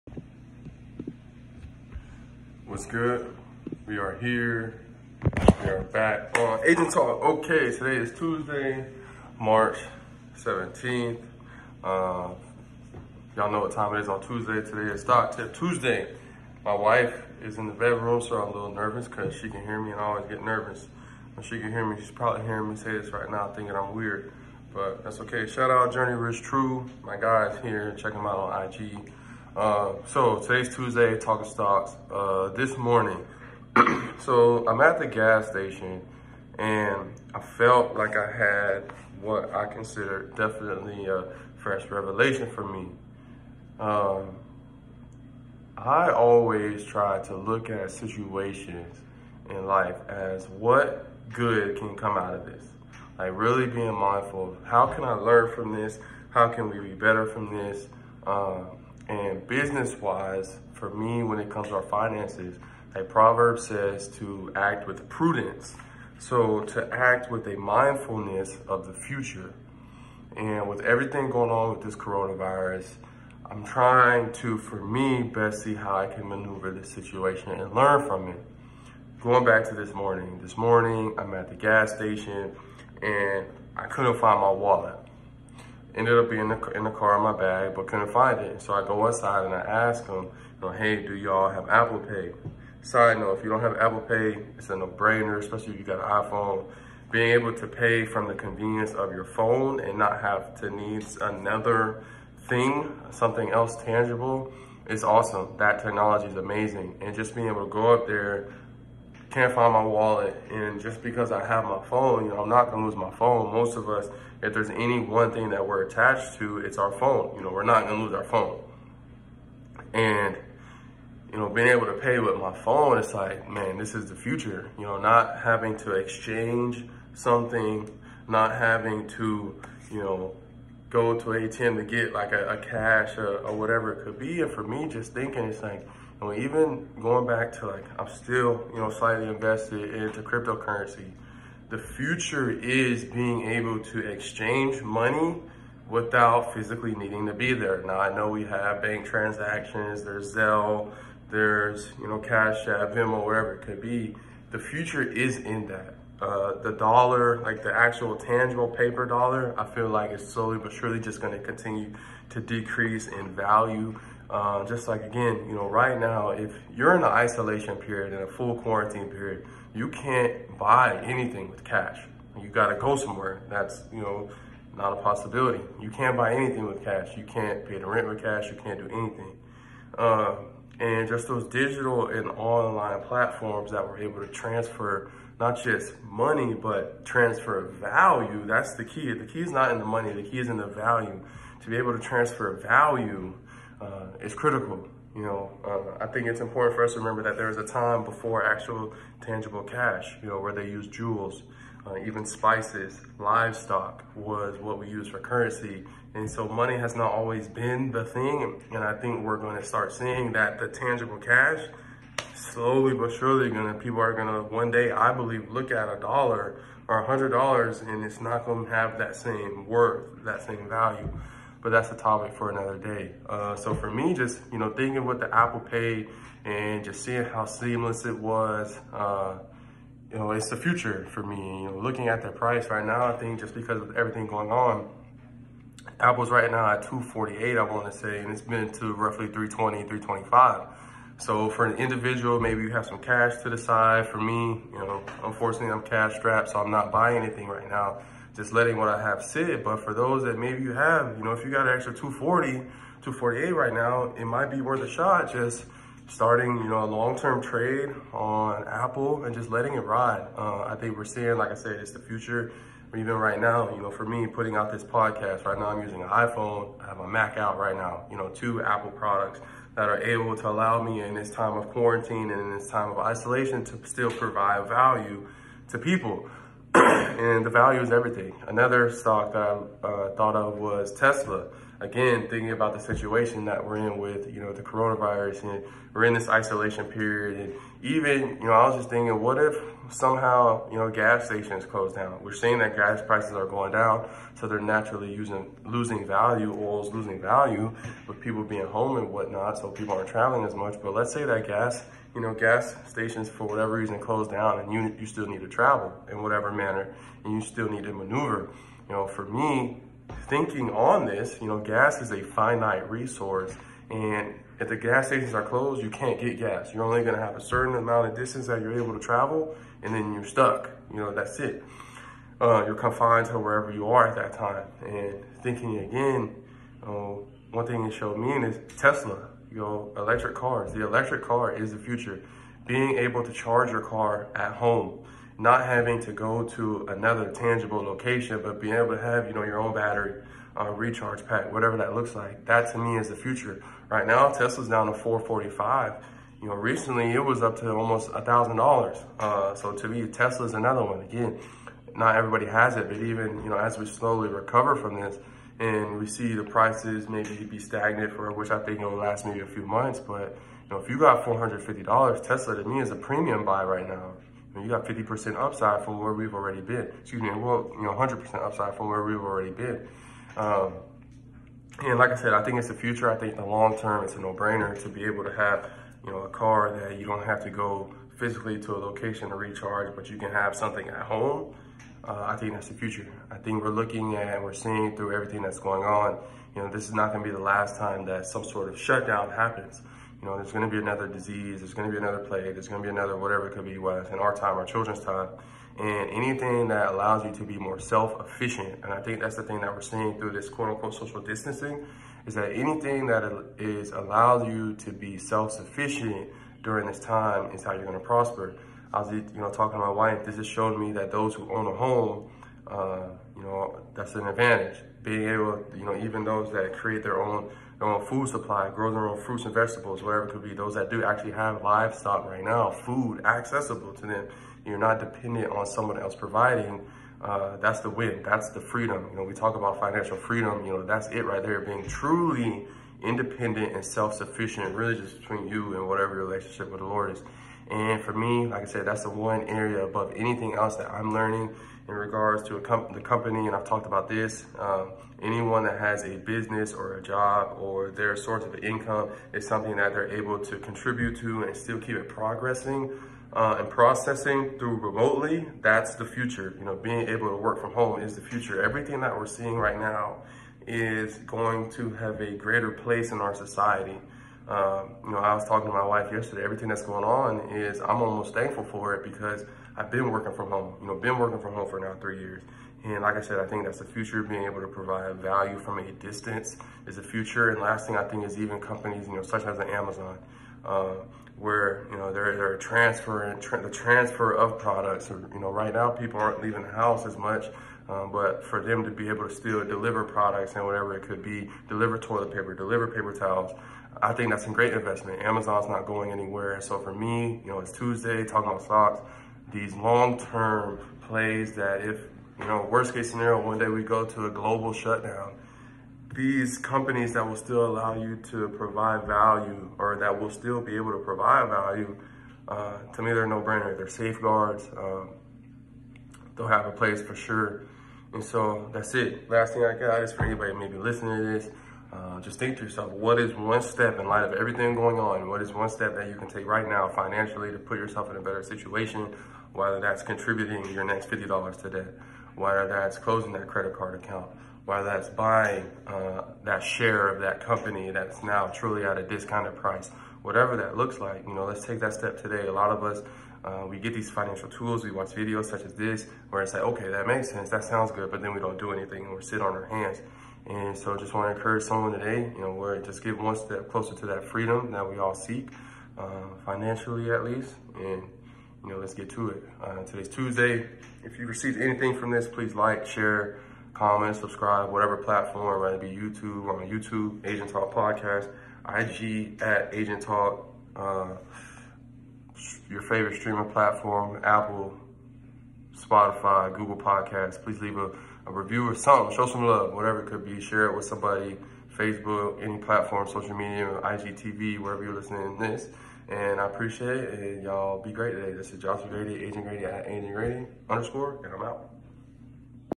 Stock Tip Tuesday - Seeing the Future Before It Happens
What's good! We are back here on Agent Talk and we UP with StockTipTuesday ! This morning I discuss what this coronavirus and quarantine situation has shown me about the potential future and how that impacts the way I am looking at a few of my investments. To be able to make money investing, we have to be able to learn from the past, and make decisions with prudence, especially when it comes to our finances. Much love!